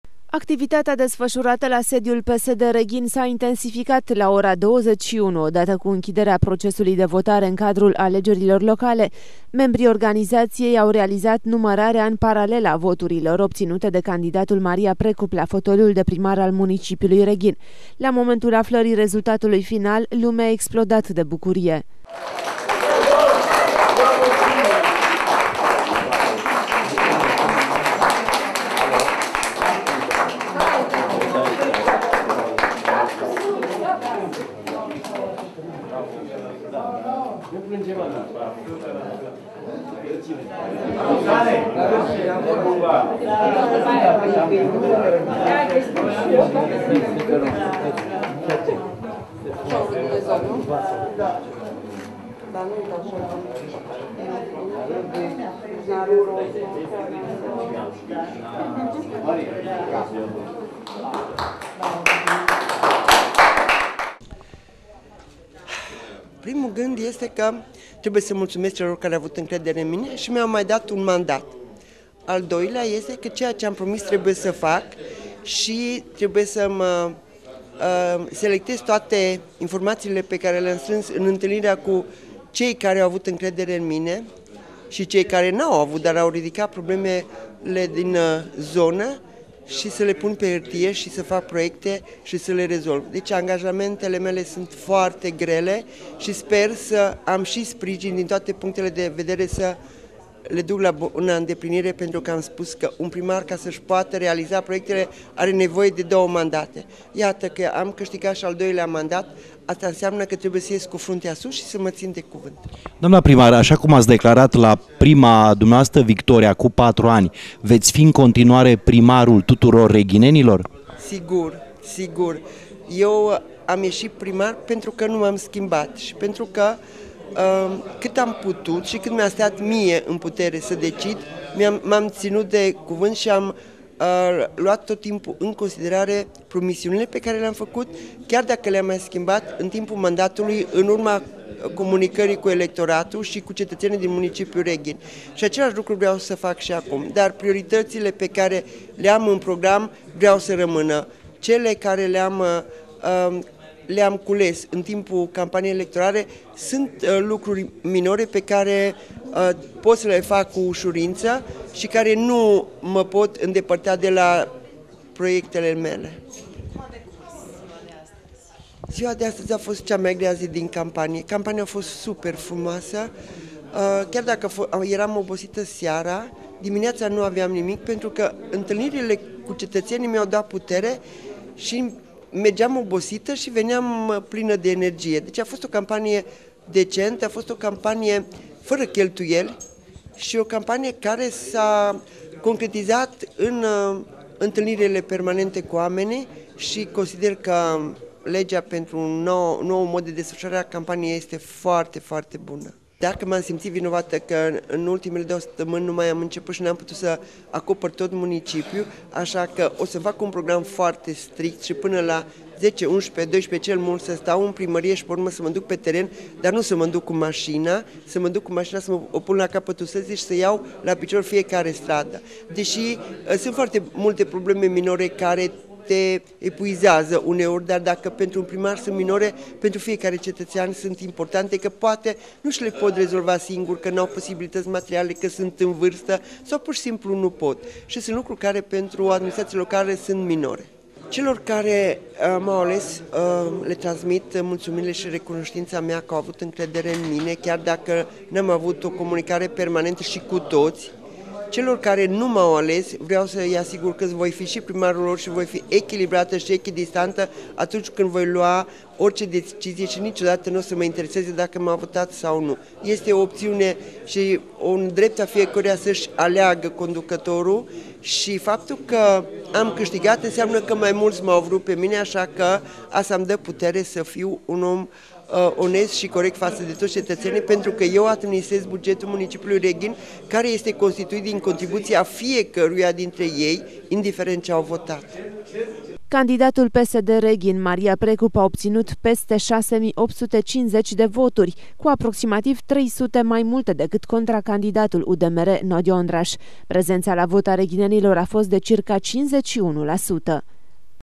CEI MAI MULŢI AU ALES MARIA PRECUP (2016 06 06)
Emoţie, urmată de bucuria victoriei, la sediul PSD Reghin, în seara zilei de duminică 5 iunie. Membrii organizaţiei locale au efectuat în paralel numărarea voturilor ţinând legătura cu reprezentanţii partidului din fiecare secţie de votare. La aflarea rezultatului final, candidatul Maria Precup a mulţumit echipei sale, dar şi celor care i-au acordat votul. A declarat la cald, pentru DAReghin TV că un primar are nevoie de cel puţin două mandate pentru a-şi realiza proiectele.